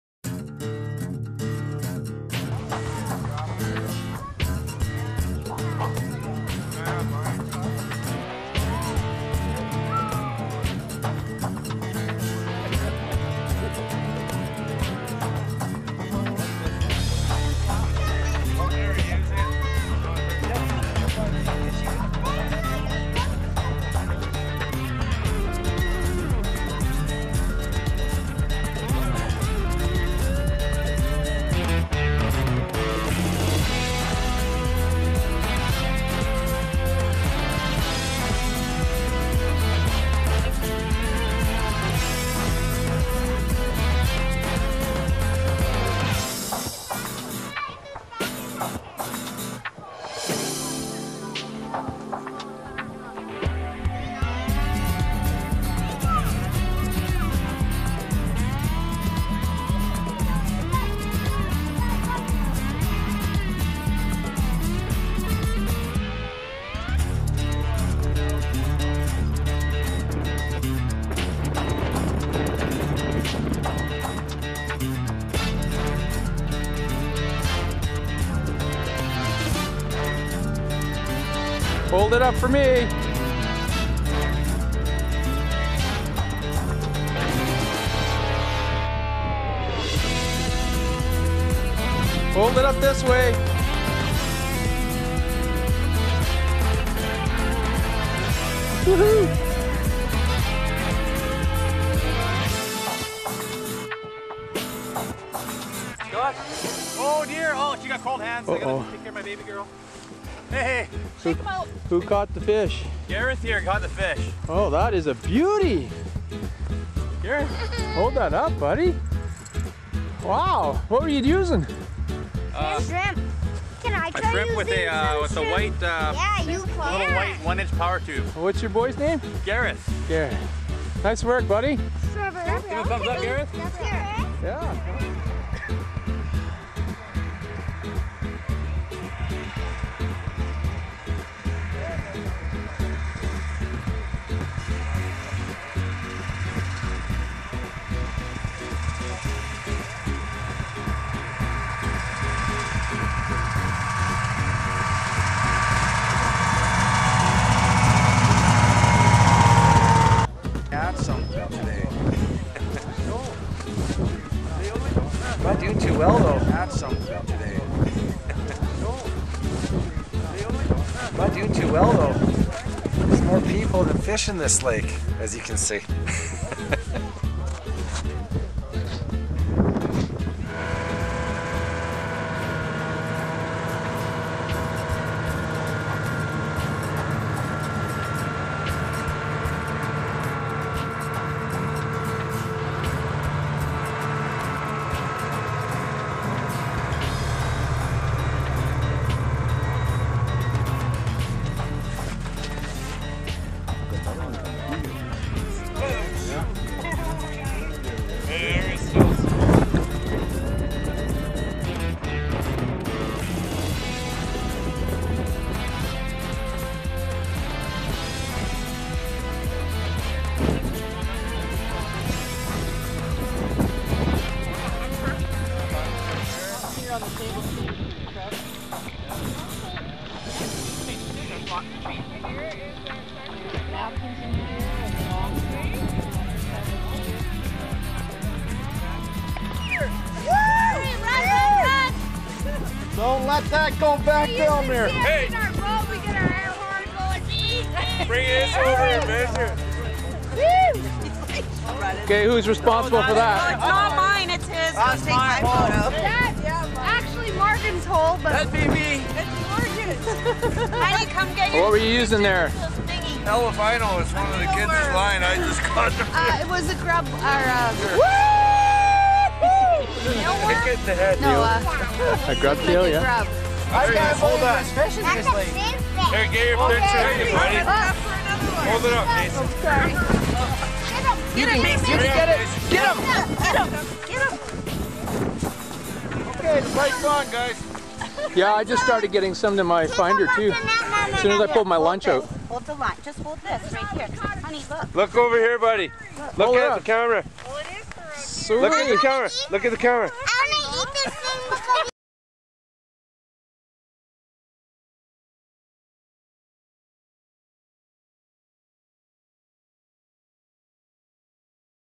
extravaganza.mpg
The first annual Ice Shanty team B.C.meet and greet. This event took place at Eena lake. B.C.